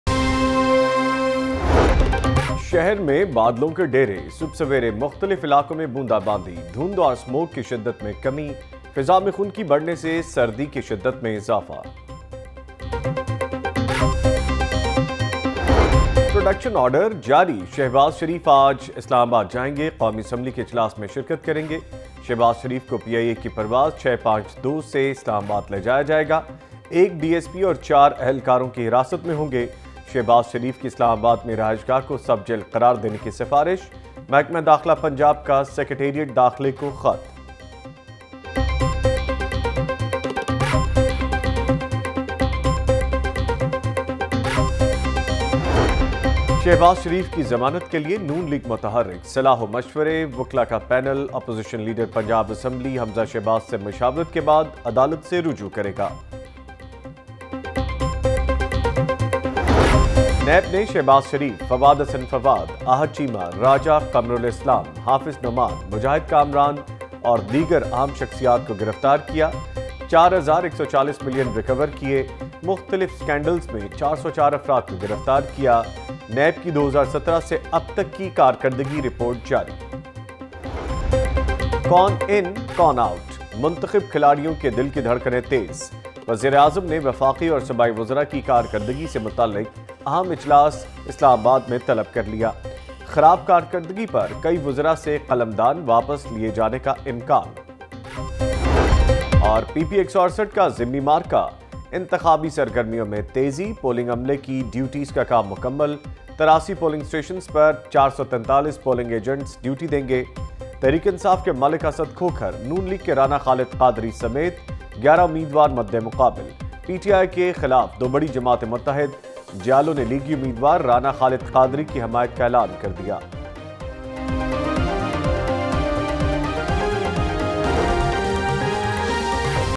09 AM Headlines Lahore News HD – 10th December 2018
Watch Latest News:
Rain drops in different areas of city, Hum Bridal Couture Week ends, Gunah Tax imposed on cigarette smoking, LHC bans demolition of Governor House’s walls, Crickter Yasir Shah breaks 82-year old record, Shehbaz Sharif moved to camp jail, Cricketer Muhammad Asif undergoes severe car accident, Imran Khan orders to demolition of Governor House, NAB arrests Former CTO Rai Ejaz, Double helmet made compulsory on pillion riding, PTI Govt completes 100- days, Qaiser Amin Butt makes significant revelations in Paragon Scandal, Khawaja Saad Rafique appears before NAB Court for bail extension in Paragon Housing Scandal, Attack at Chines consulate Karachi, Lahore among worst polluted cities ,Gas shortage prevails in Lahore, , Petrol prices witness sharp increase, Poultry prices witness sharp increase, Dry Fruit prices increases, , Immense traffic jam in Lahore, Scuffle breaks out between locals and traffic wardens, Dollar prices witness sharp increase, Oil tanker collides at Waris Meer underpass and much more.